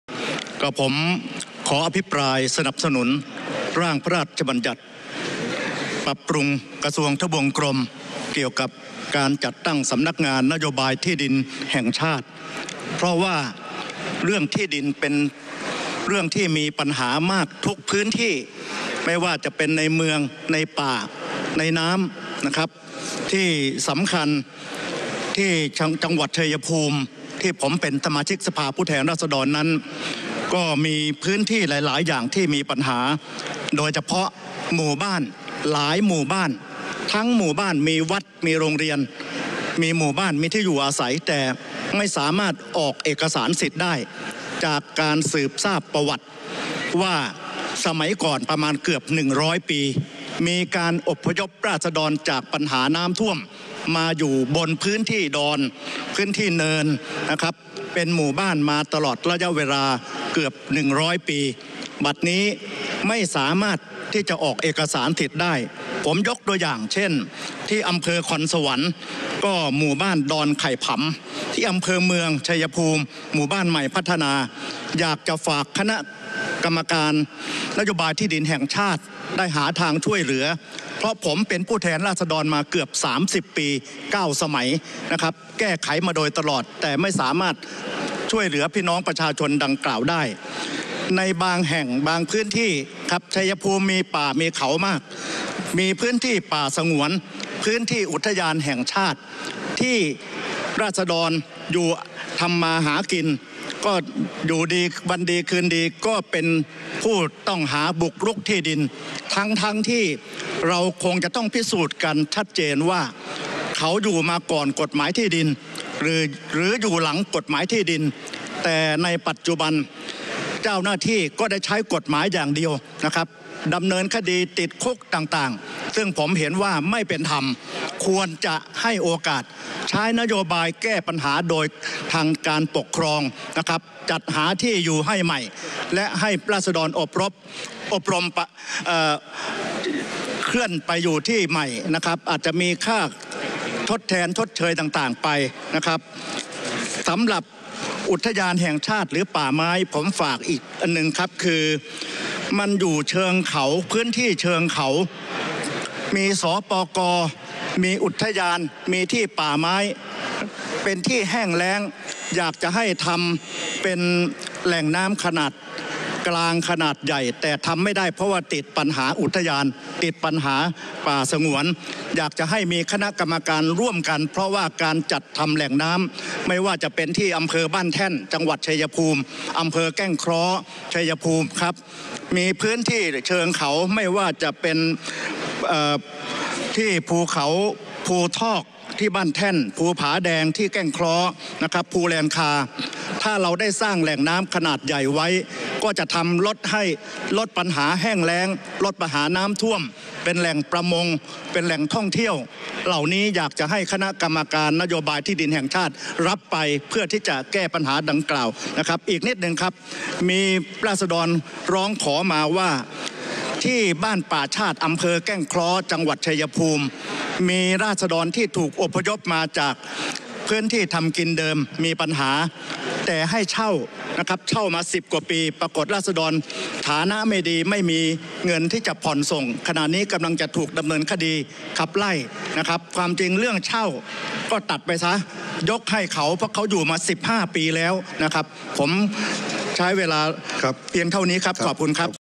นพ.สุรวิทย์ คนสมบูรณ์ เรื่องด่วน ร่างพระราชบัญญัติปรับปรุงกระทรวง ทบวง กรม (ฉบับที่..) พ.ศ. ...
นพ.สุรวิทย์ คนสมบูรณ์ ส.ส.ชัยภูมิ พรรคเพื่อไทย
ระเบียบวาระการประชุมร่วมกันของรัฐสภา
เรื่องด่วน ร่างพระราชบัญญัติปรับปรุงกระทรวง ทบวง กรม (ฉบับที่..) พ.ศ. ...
การประชุมสภาผู้แทนราษฎร วันที่ 4 สิงหาคม 2563